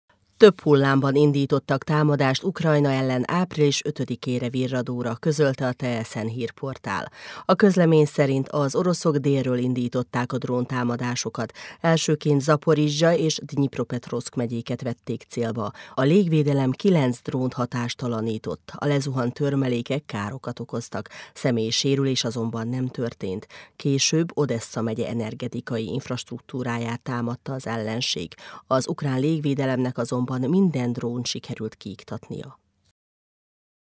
Több hullámban támadták Ukrajnát péntekre virradóra
Több hullámban indítottak támadást Ukrajna ellen április 5-re virradóra – közölte a tsn.ua hírportál.   A közlemény szerint az oroszok délről indították a dróntámadásokat.  Elsőként Zaporizzsja és Dnyipropetrovszk megyéket vették célba. A légvédelem 9 drónt hatástalanított. A lezuhant törmelékek károkat okoztak. Személyi sérülés nem történt. Később Odessza megye energetikai infrastruktúráját támadta az ellenség. Az ukrán légvédelemnek azonban minden drónt sikerült kiiktatnia.